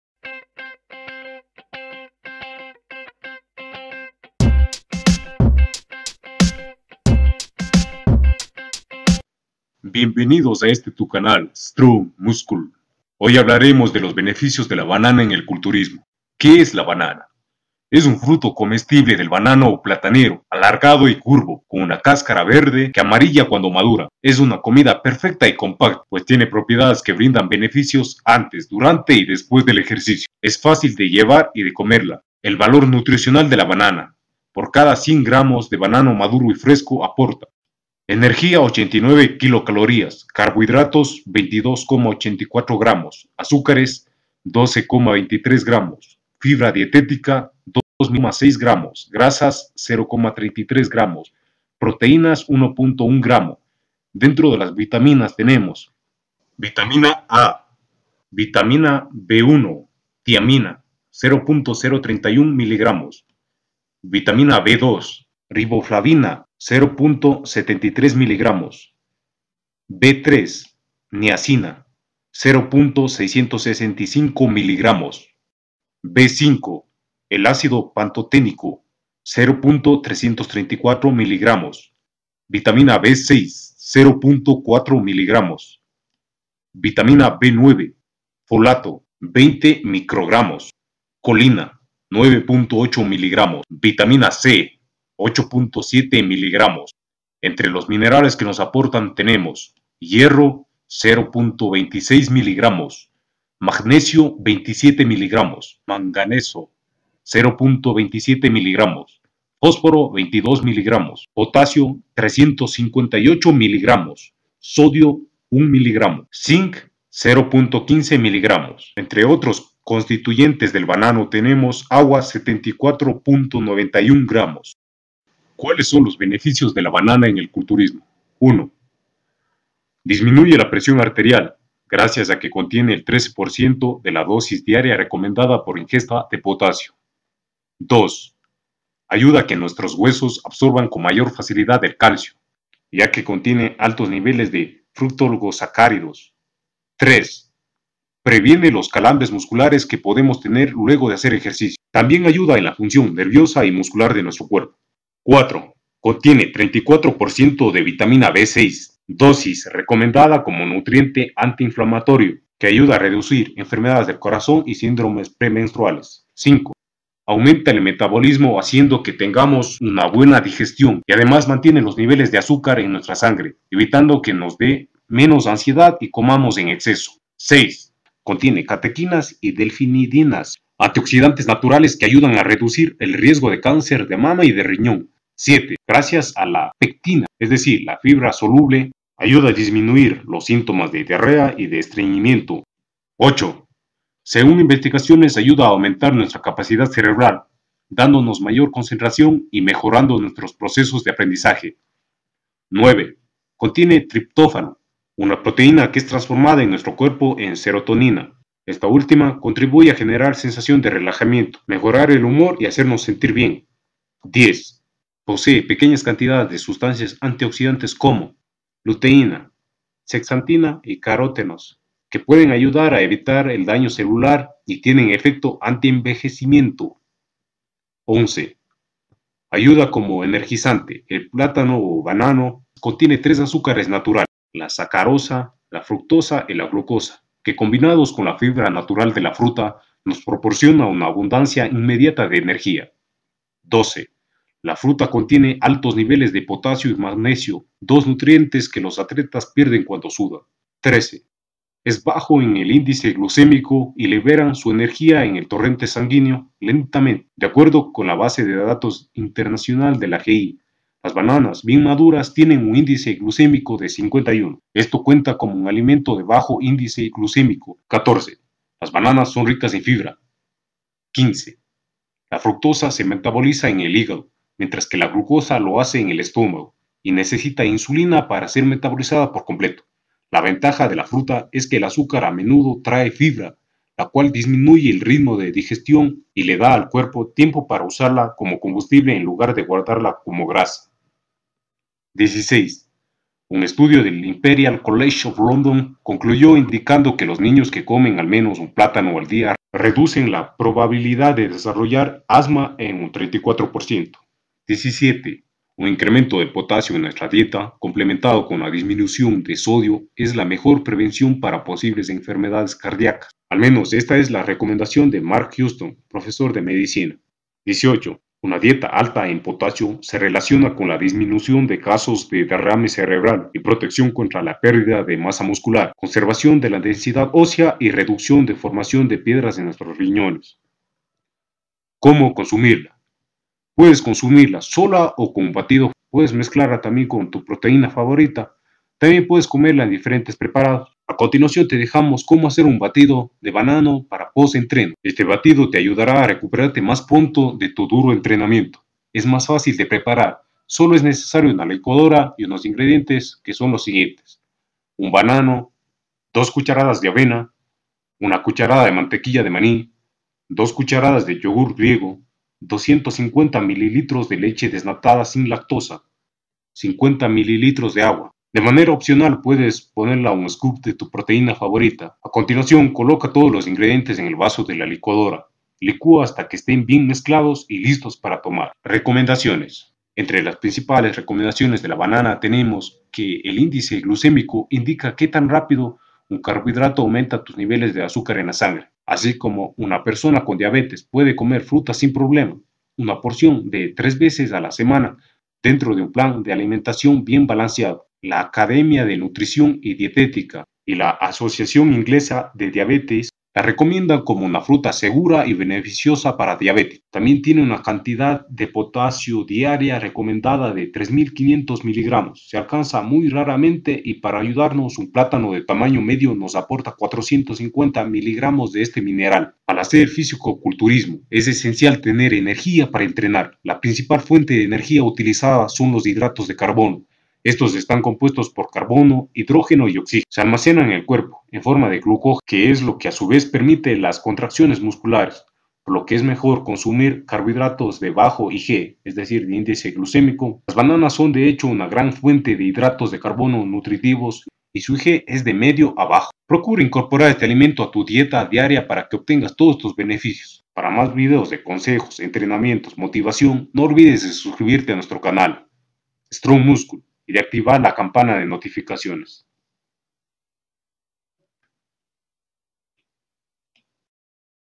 BENEFICIOS DE LA BANANA EN EL CULTURISMO / AUMENTAR MASA MUSCULAR
QUE ES LA BANANA.- Es un Fruto comestible del banano o platanero, alargado y curvo, con una cáscara verde que amarilla cuando madura.

Es una comida perfecta y compacta, pues tiene propiedades que brindan beneficios antes, durante y después del ejercicio, es fácil de llevar y comerla.
VALOR NUTRICIONAL DE LA BANANA miralo en el vídeo, así como un buen batido para aumentar masa muscular.

REVISA VÍDEOS DE INTERÉS